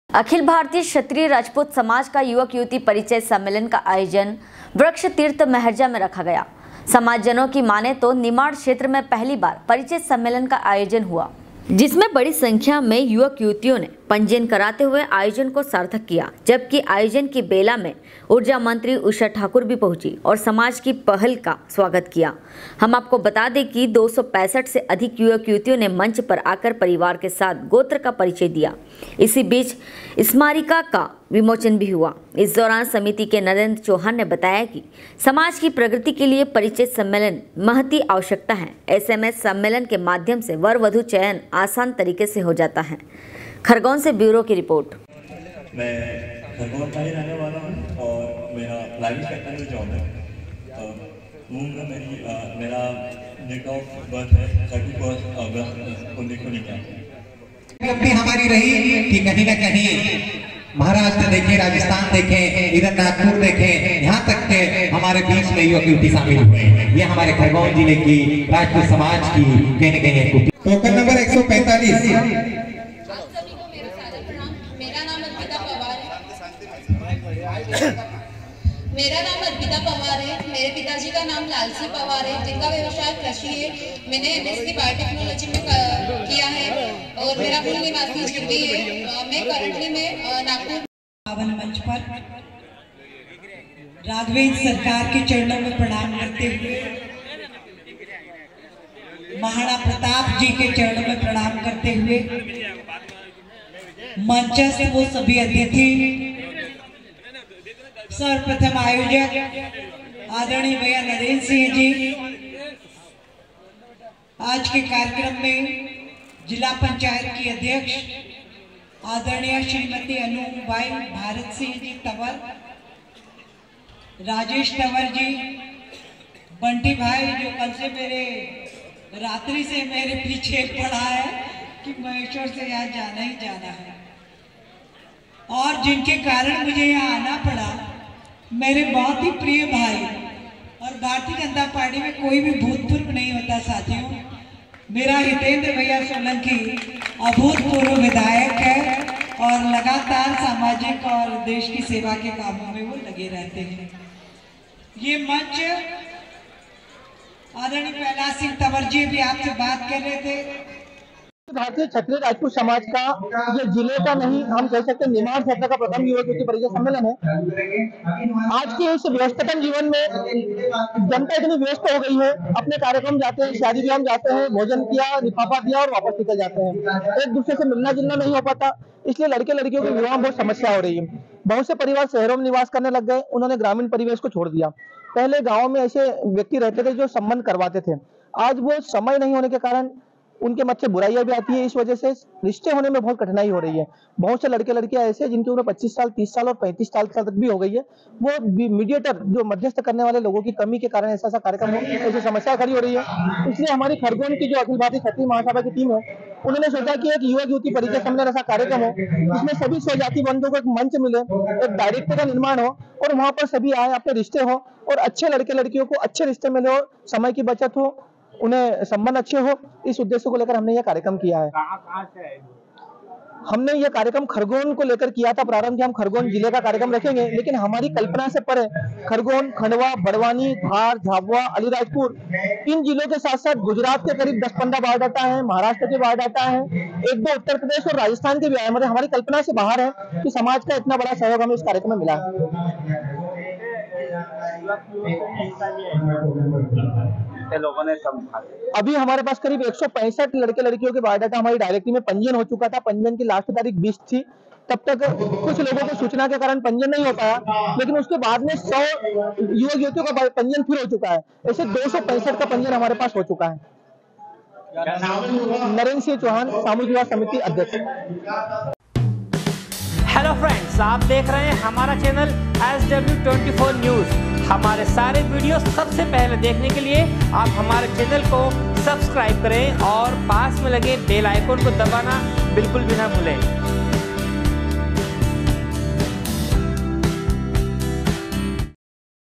अखिल भारतीय क्षत्रिय राजपूत समाज का युवक युवती परिचय सम्मेलन का आयोजन वृक्षतीर्थ मेहरजा में रखा गया।
अखिल भारतीय क्षत्रिय राजपूत समाज का युवक युवती परिचय सम्मेलन का आयोजन वृक्षतीर्थ मेहरजा में रखा गया। समाजजनों की माने तो निमाड़ क्षेत्र में पहली बार परिचय सम्मेलन का आयोजन हुआ खरगोन से ब्यूरो की रिपोर्ट।